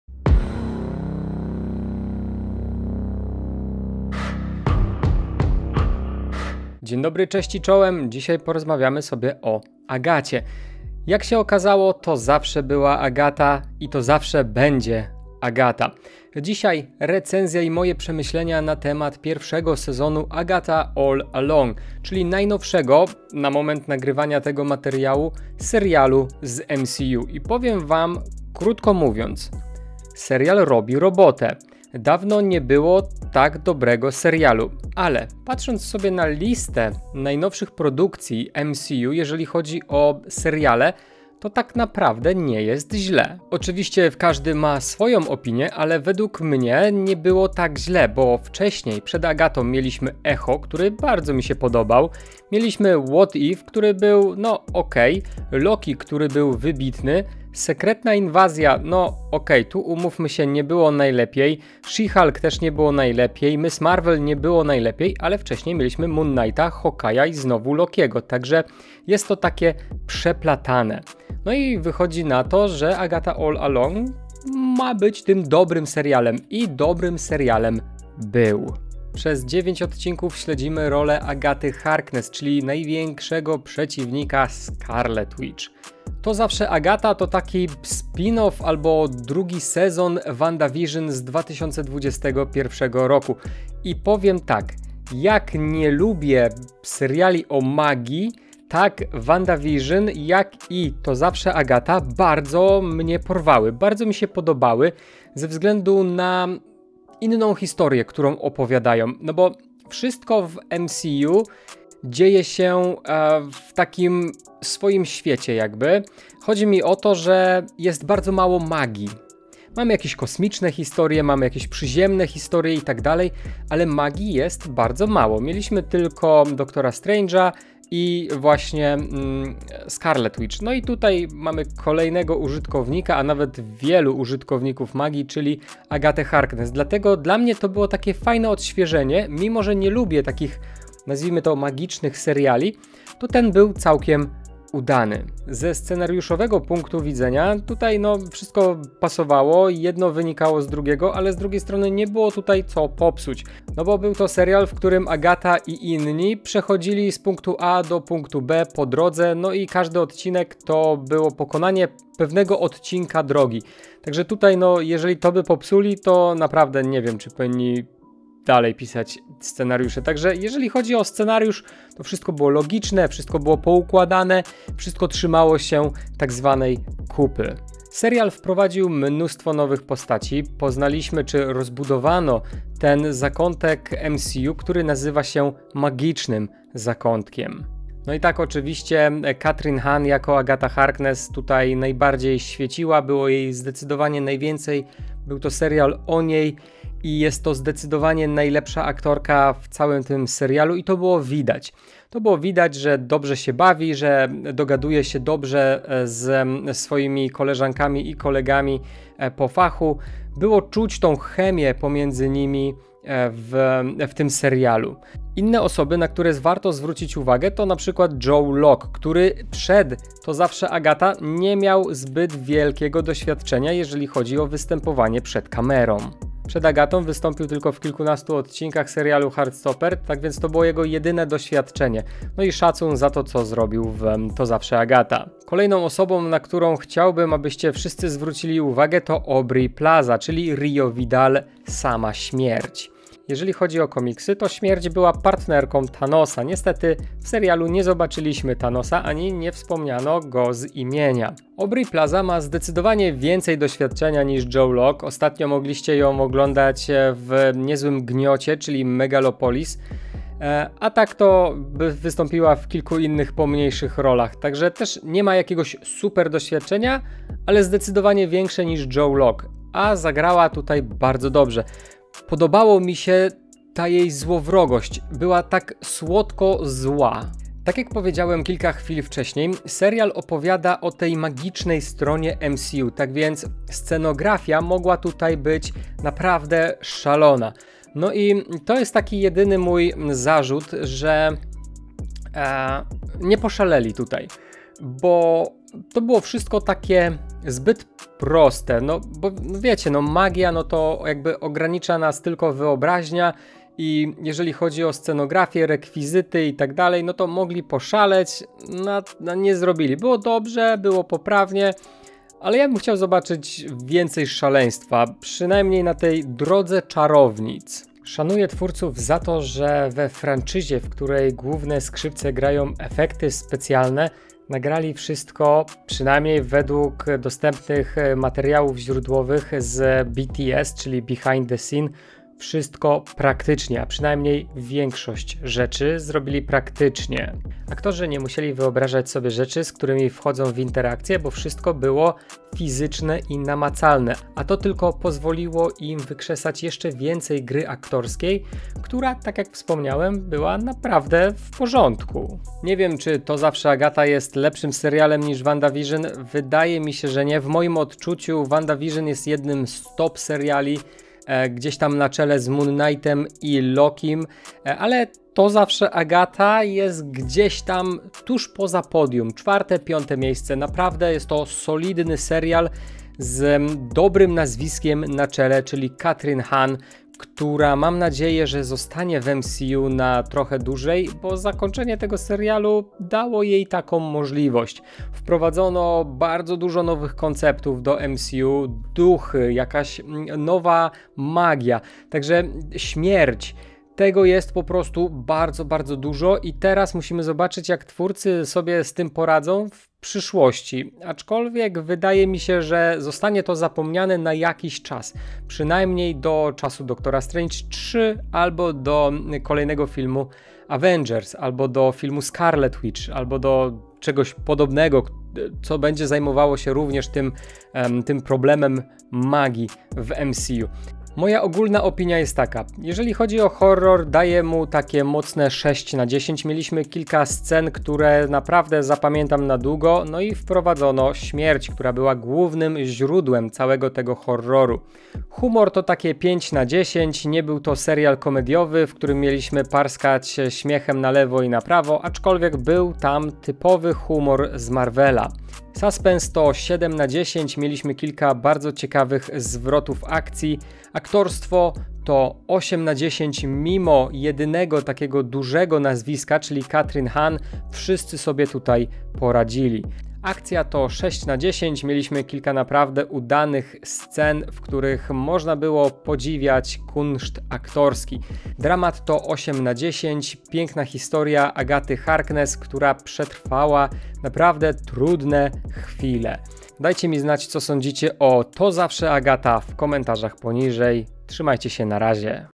To jednak zawsze była Agata - recenzja serialu
Halfish - To jednak zawsze była Agata - recenzja serialu

Muzyka: biblioteka YT

Jeśli chcesz mieć SUPER bonusy i oglądać nowe filmy przed wszystkimi innymi włącz wspieranie za pomocą tego linku

00:00 Wstęp
00:06 recenzja
07:00 werdykt